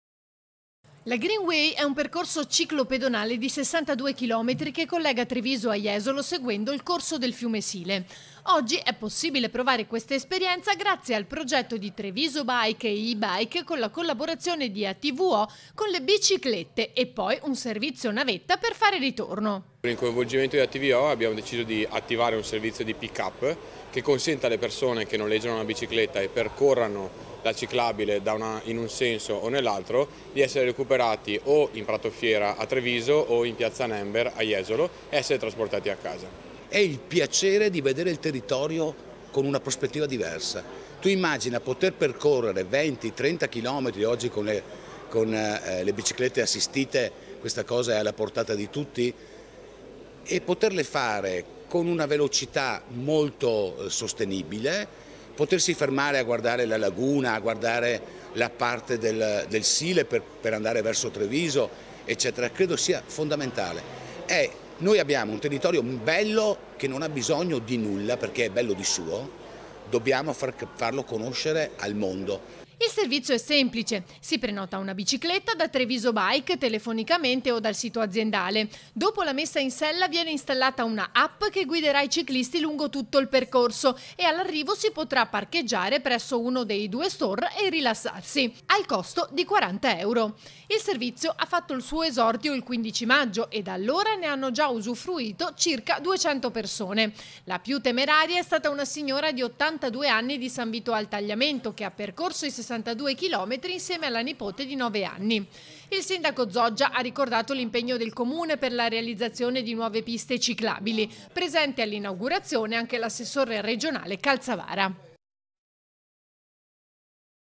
DA JESOLO A TREVISO CON LA BICI E IL BUS | 09/07/2021
A3 NEWS Treviso 09/07/2021 - JESOLO - E’ stato presentato questa mattina a Jesolo il progetto Bus & Bike per raggiungere Treviso da Jesolo con le biciclette o un servizio navetta.   || La Greenway è un percorso ciclo-pedonale di 62 km che collega Treviso a Jesolo seguendo il corso del fiume Sile. Oggi è possibile provare questa esperienza grazie grazie al progetto di Treviso.bike e Ebike, con la collaborazione di Atvo, con le biciclette e poi un servizio navetta per il ritorno.Il servizio è semplice: si prenota una bicicletta da Treviso.bike telefonicamente o dal sito, dopo la messa in sella, viene installata una app che guiderà i ciclisti lungo tutto percorso e all’arrivo si può parcheggiare presso uno dei due store e rilassarsi. Al costo di 40 euro. Il servizio ha fatto il suo esordio il 15 maggio e da allora ne hanno già usufruito circa 200 persone. La più temeraria è stata una signora di 82 anni di San Vito al Tagliamento che ha percorso i 62 km insieme alla nipote di 9 anni. Il sindaco Zoggia ha ricordato l’impegno del Comune per la realizzazione di nuove piste ciclabili, presente all’inaugurazione anche l’assessore regionale Calzavara. - Intervistati GIOVANNI DAL POZ (TREVISO.BIKE), FABIO TURCHETTO (PRESIDENTE ATVO) (Servizio di Ilaria Marchiori) - Segui Antenna Tre anche sul digitale terrestre! Visita il sito